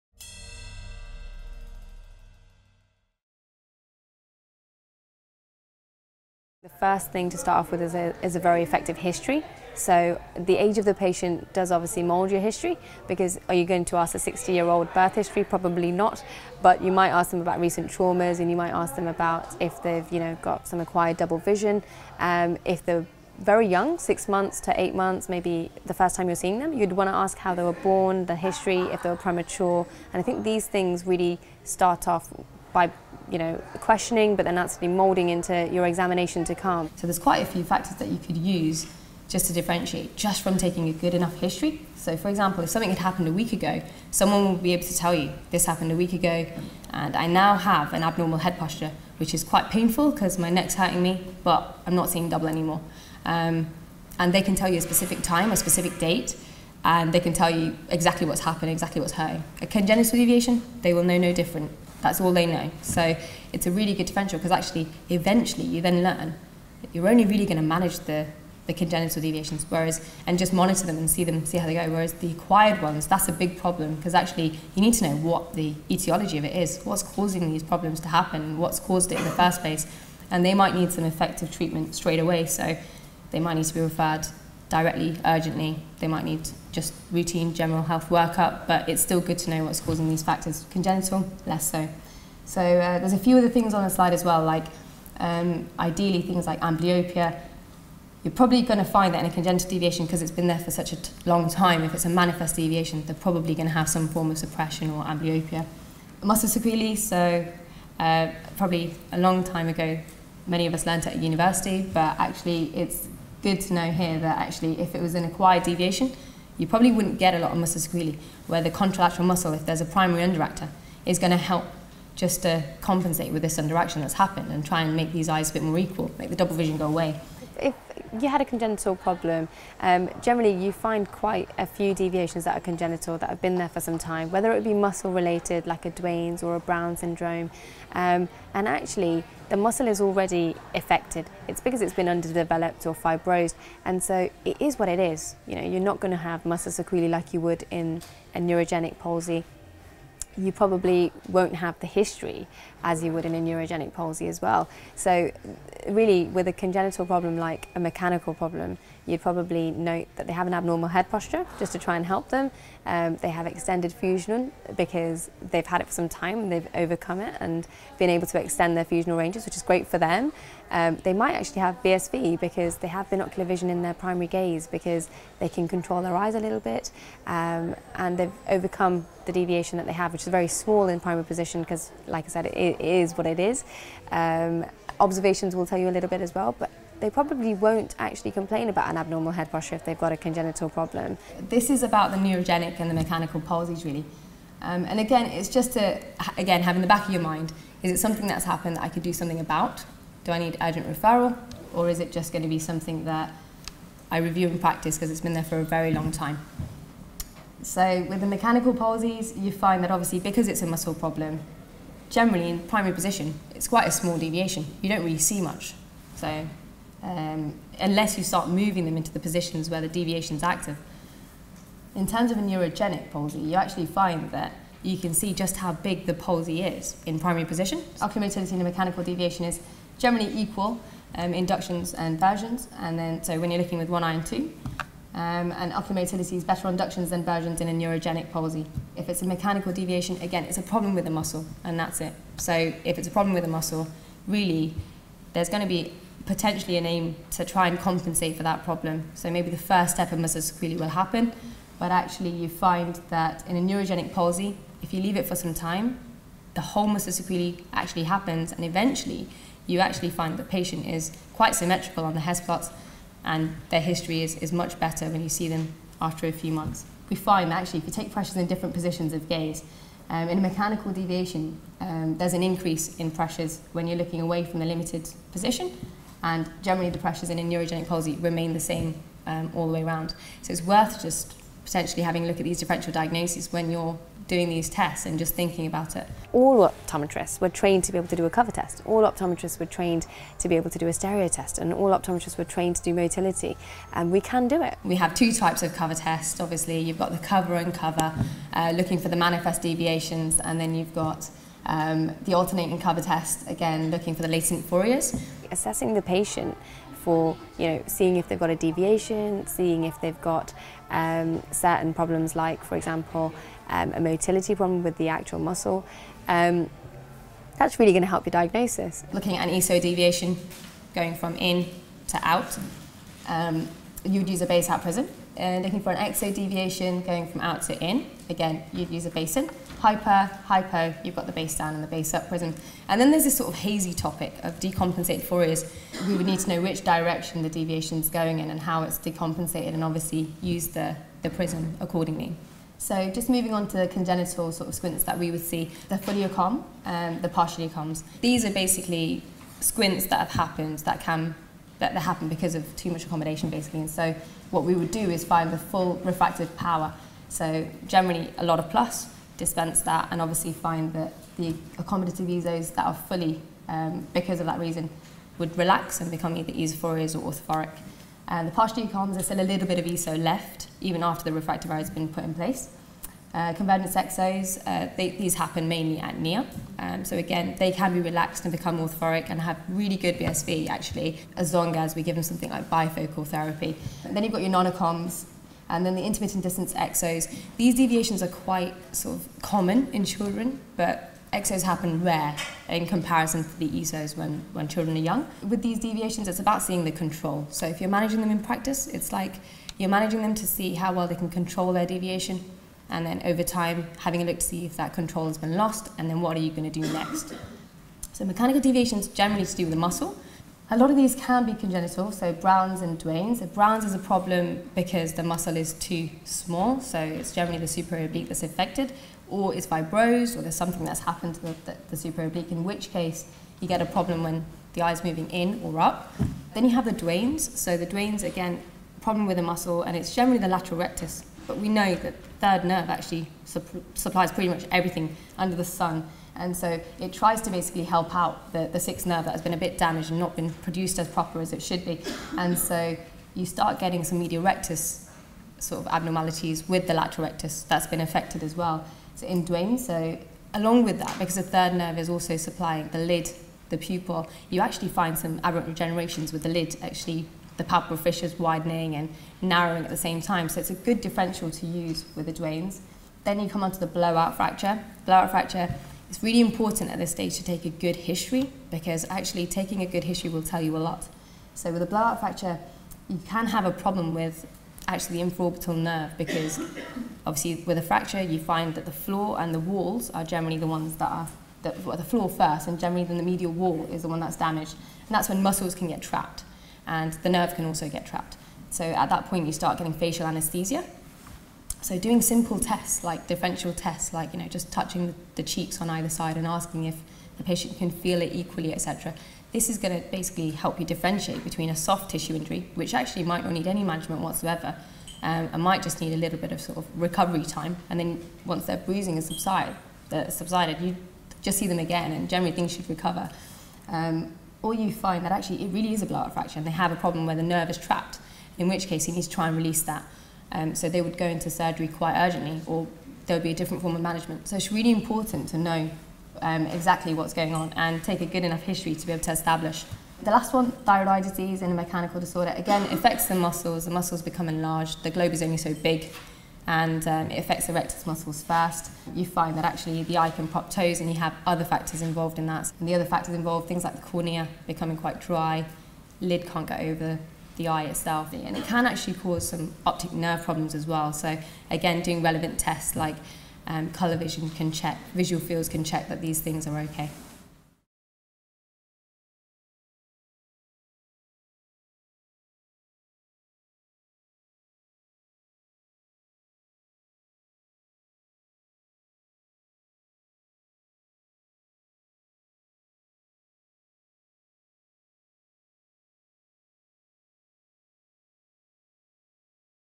What are the steps to a binocular vision assessment?
Optometrist and Orthopitist, Zarna Dasani, guides us through managing a binocular vision assessment